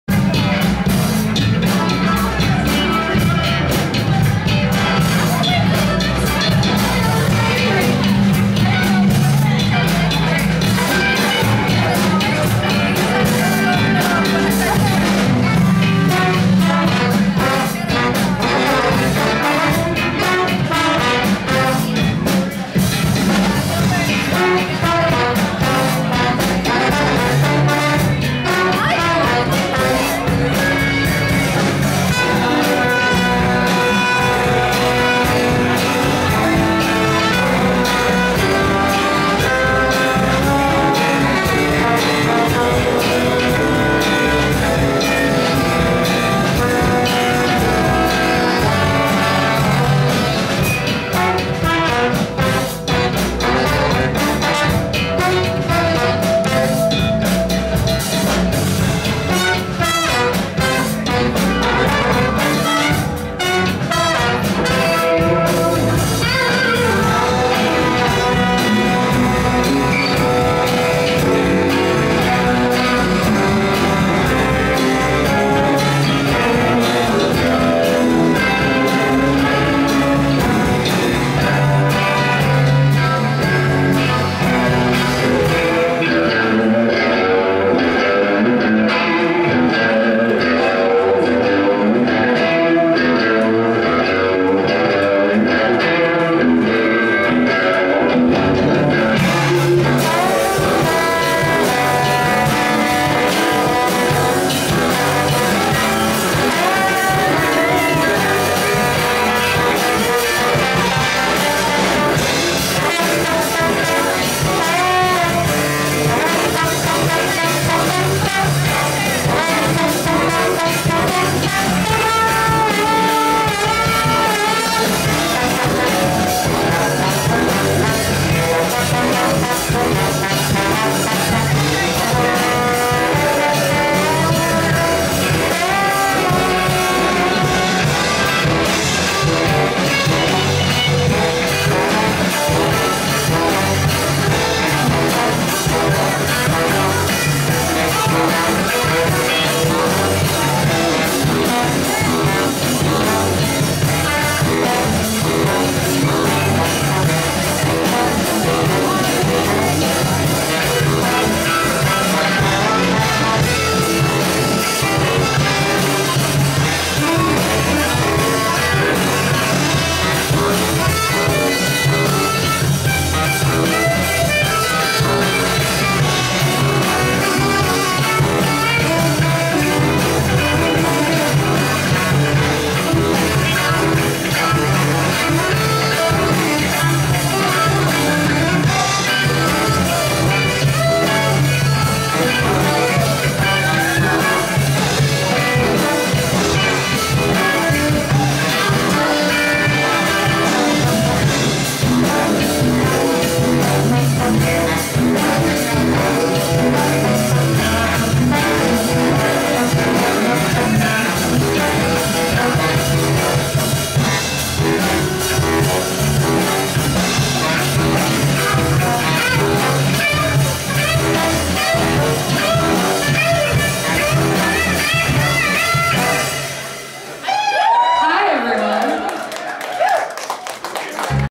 FamilyOutingAtRyleigh'sOysterMountVernon00098
FAM(ily) Outing: Live Music & Art / Meet & Greet ft. Fractal Cat

Hosted by Firehouse Arts & Music

Saturday, February 25 at 8 PM - 1 AM
Feb 25 at 8 PM to Feb 26 at 1 AM
pin

The Basement at Ryleigh's in Mt Vernon
1225 Cathedral Street, Baltimore, Maryland 21201

(tribute performance for John Suntiger including video content by Suntiger Visuals and choreographed dance by Lindsey Golden)

*Featuring an Art talk by Will Shanklin of the Baltimore Art Monkeys

*Local artists including:
Richard Best, Dave Nyquist, Alyse Firefly, Sophie Spinklemorph, Seth Milder, Scott Frias, Benjamin Joseph Jancewicz & Sophia Sperling

*Hoop/ Flow Performance by Lisa Eibner

*Kaitlyn Furey will be providing tarot readings

*Local vendors including:
Tree Frog Hot Sauce, Caitlin Harris & Hanged Man Arts